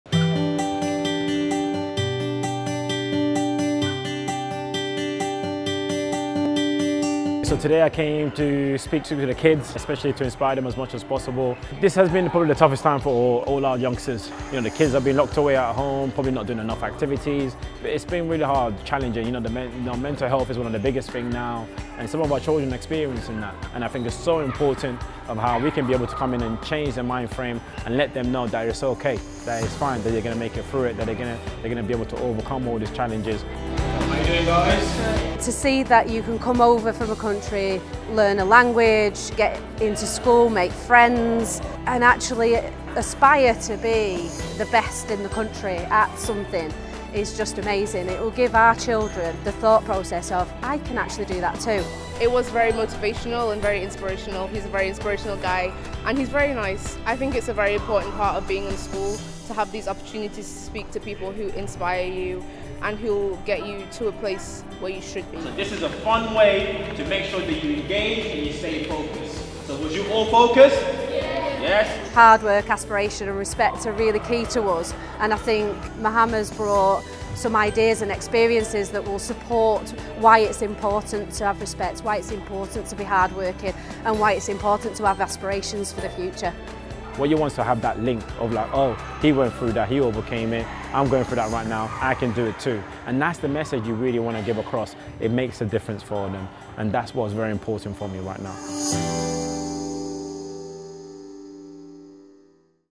Mahama Cho visits Cedar Mount Academy in Mancs Nov 2021
Fresh from their heroics in Tokyo, British Olympians and Paralympians have been lifting the spirits of a generation of young people through a nationwide programme of school visits.

Children’s charity the Youth Sport Trust is working with UK Sport, British Olympic Association and British Paralympic Association, to build on the inspiration and positivity children felt from watching sporting stars compete at this summer’s Olympic and Paralympic Games.

Hear from Mahama Cho about his visit to Cedar Mount Academy in Manchester.